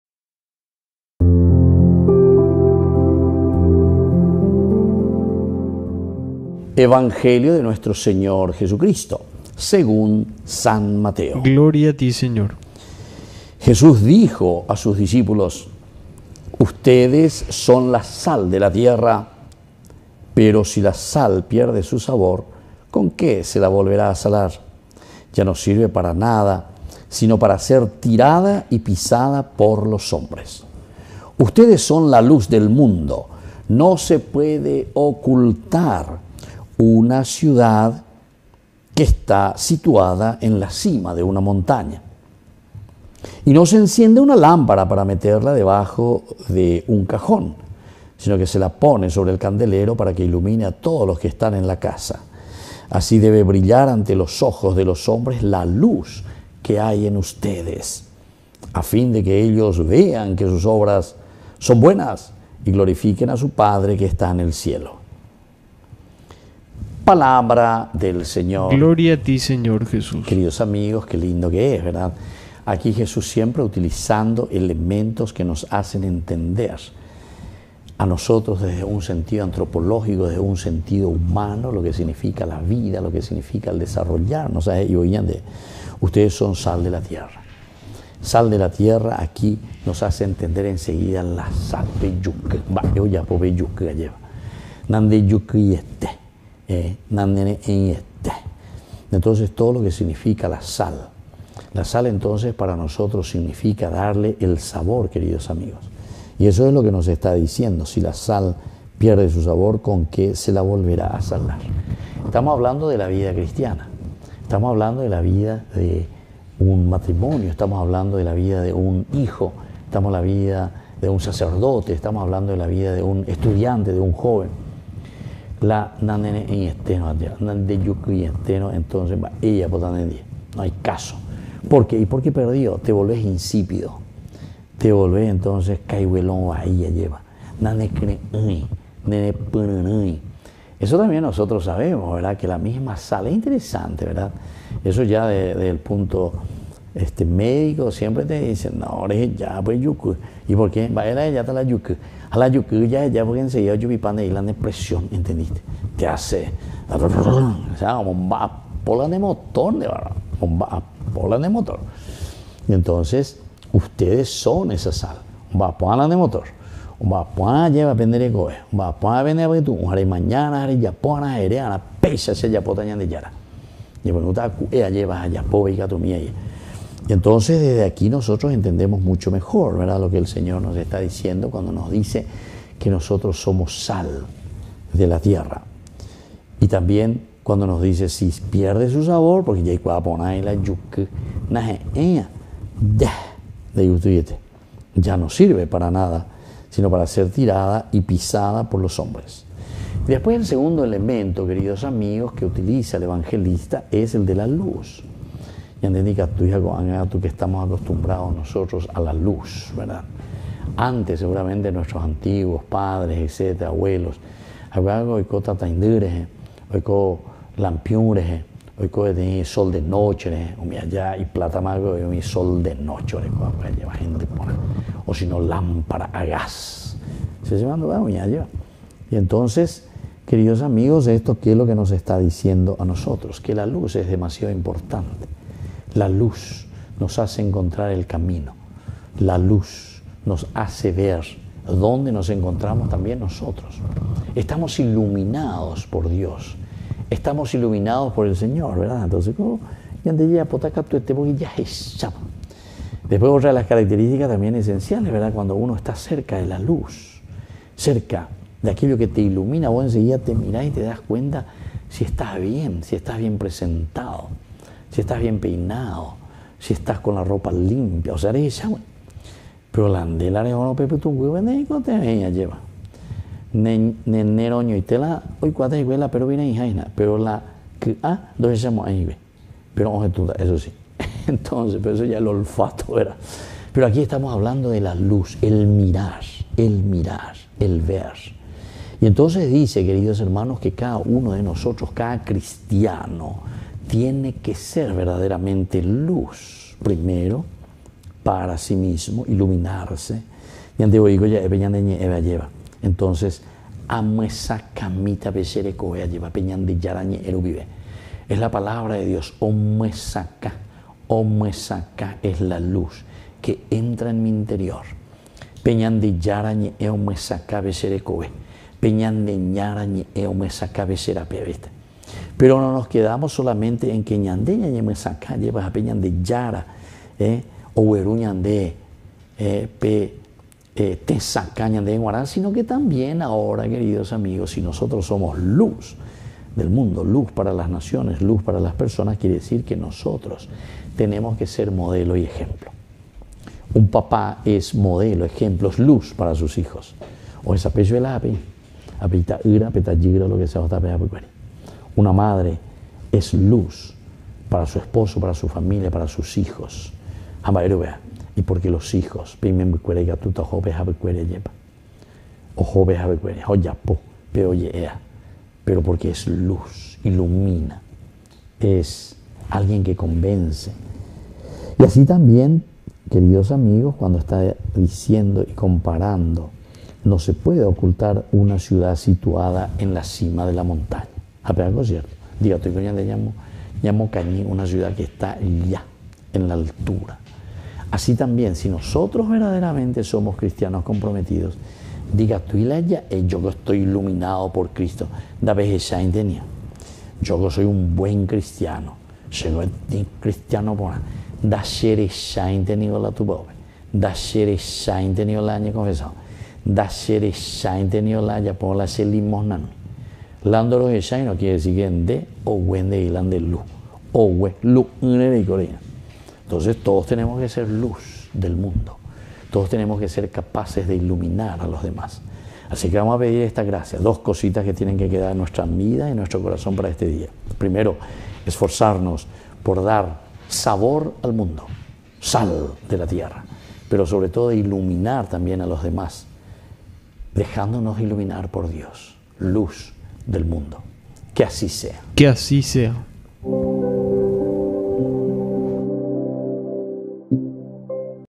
HOMILIA MARTES 13 DE JUNIO DEL 2023
Santo Evangelio según San Mateo 5, 13-16:
"Ustedes son la luz del mundo y sal de la tierra."